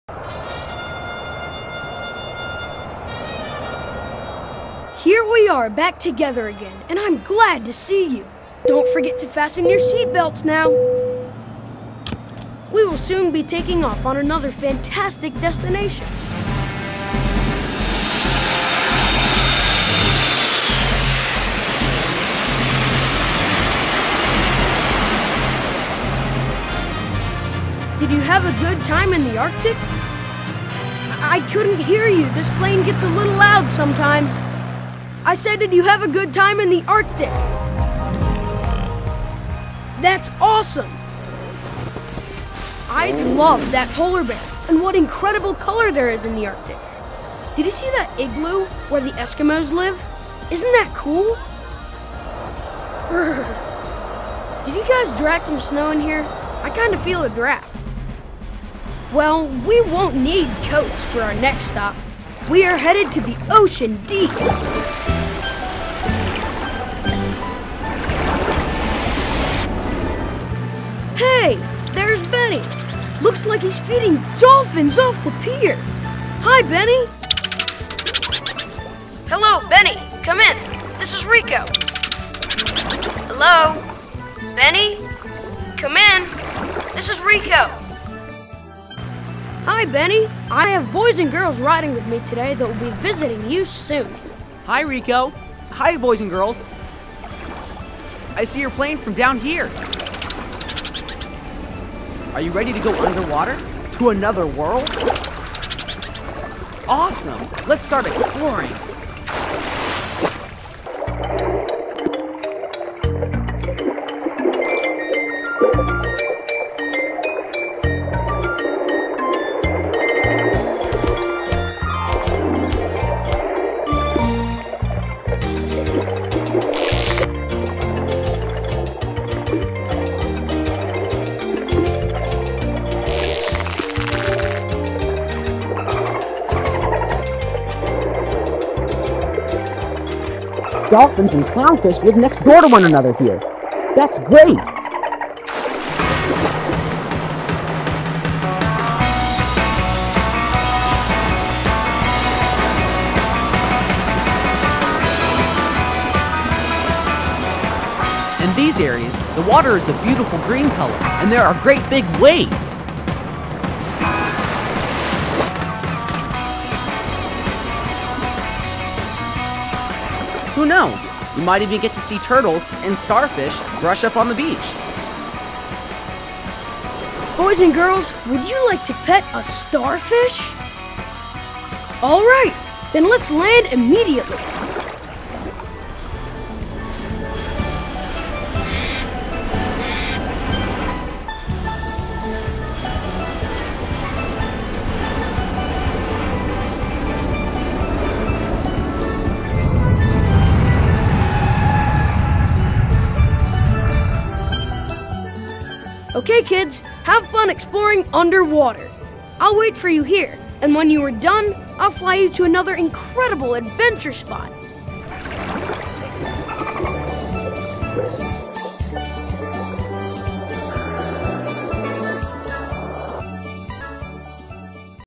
#3 AirVentures: Travel to the Ocean Deep
Benny gives you a tour of the wonderful things under the surface of the Ocean. This video is featured in ESC Region 19 Head Start's IntelliZeum.